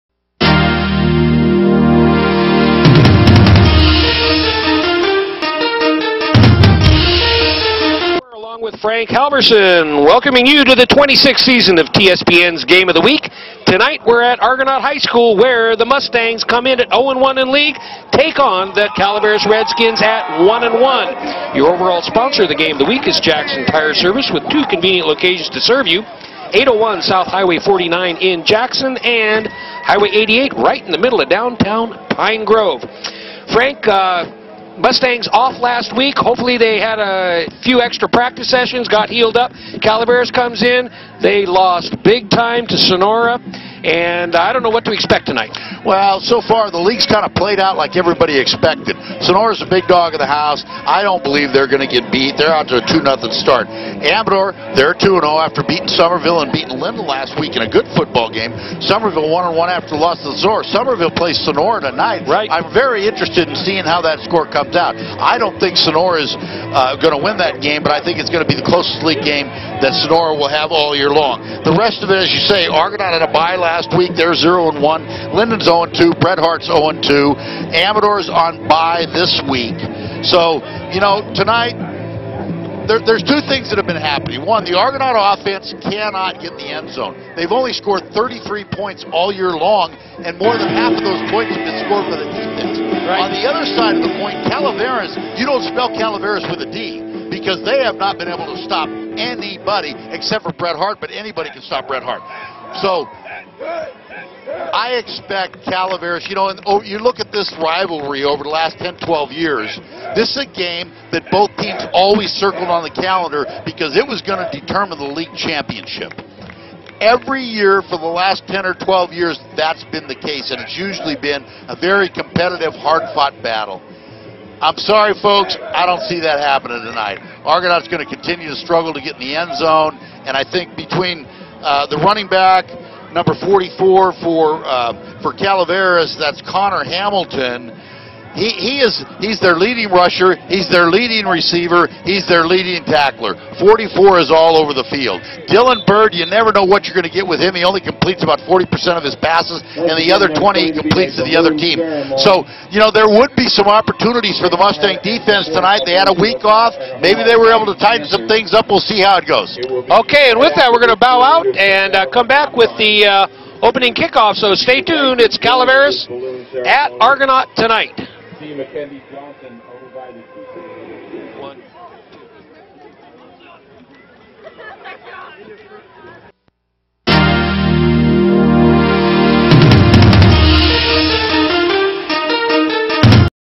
Calaveras vs. Argonaut Pregame TSPN Game of the Week
Jim Vinciguerra and Frank Halvorson preview this week's game between the Calaveras Redskins and the Argonaut Mustangs.